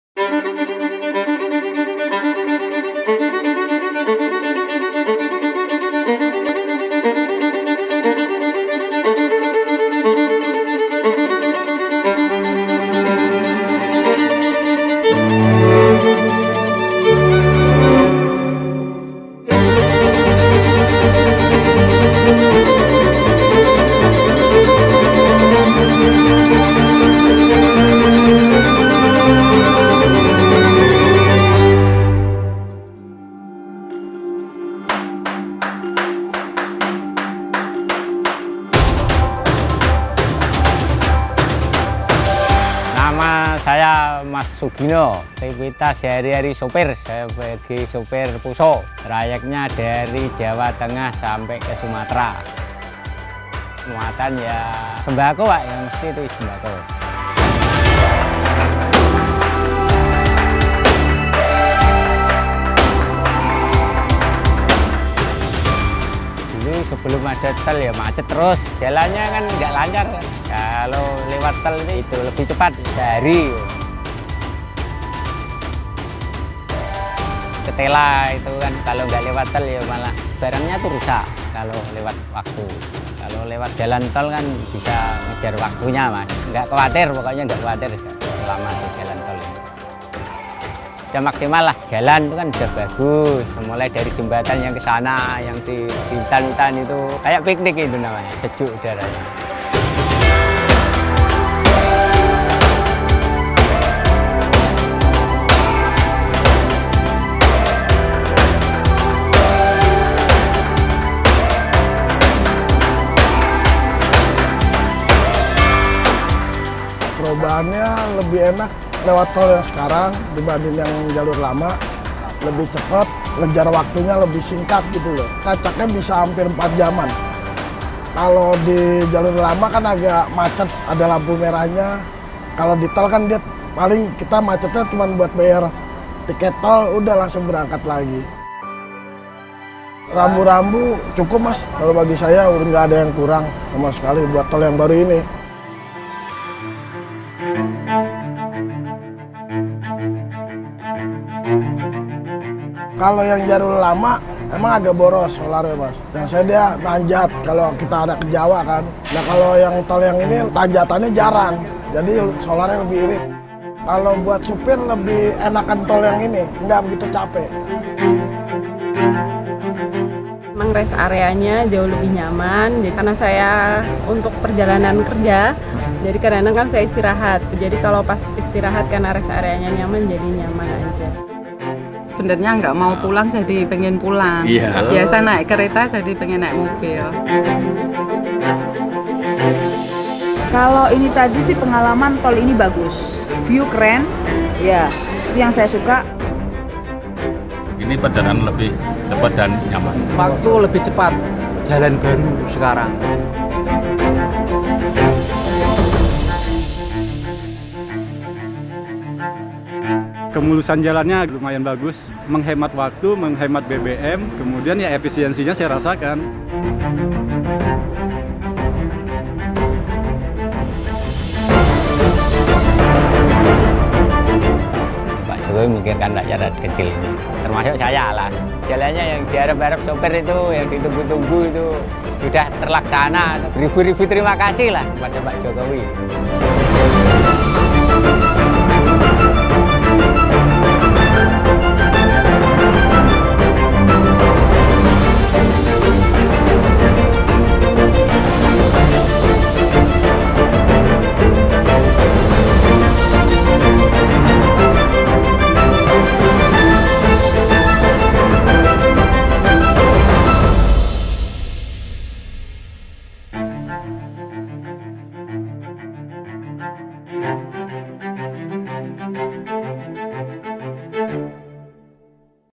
[MajuBer5ama] Pembangunan Infrastruktur Trans Jawa Tingkatkan Akses ke Destinasi Wisata
Hai SobatWisata, pastinya kalian sudah tidak asing lagi dengan keberadaan Tol Trans Jawa yang dibangun oleh Pemerintah melalui Kementerian Pekerjaan Umum dan Perumahan Rakyat dalam kurun waktu lima tahun terakhir. Jalan tol sepanjang kurang lebih 1000 kilometer ini menghubungkan sejumlah destinasi yang dilaluinya. Pembangunan jaringan jalan tol tentunya diharapkan dapat memberi manfaat kepada masyarakat.

Seperti yang disampaikan penerima manfaat dalam video, kondisi jalan yang bagus, bebas hambatan, lengkap dengan rambu-rambu jalan dan pemandangan yang indah, serta ditambah dengan adanya rest area yang nyaman nyatanya telah mempermudah aksesibilitas masyarakat. Selain itu, seperti pesan Presiden Jokowi, tersambungnya jalan tol ini diharapkan mampu meningkatkan konektivitas industri pariwisata dan mempermudah akses menuju destinasi wisata yang nantinya berdampak pada perputaran perekonomian masyarakat.

SobatWisata, kalian sudah coba lewatin Tol Trans Jawa kan? Kalau belum, buruaan.